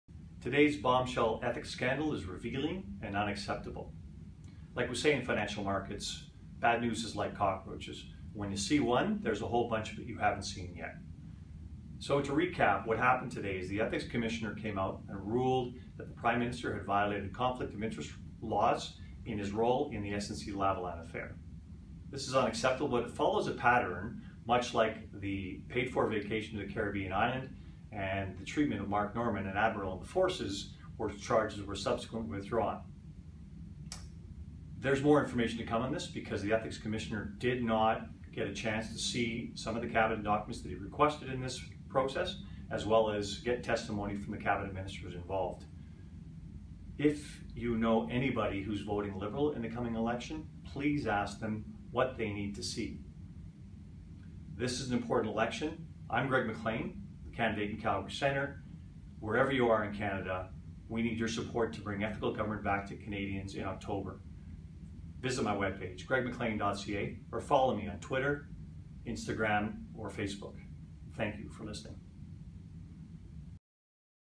Greg McLean on Prime Minister Trudeau's Ethics
The Ethics Commissioner has ruled that - for the second time - the Prime Minister has broken the law by violating the Conflict of Interest Act: this time with respect to the SNC Lavalin scandal. Is this the person we want to be Canada's Prime Minister?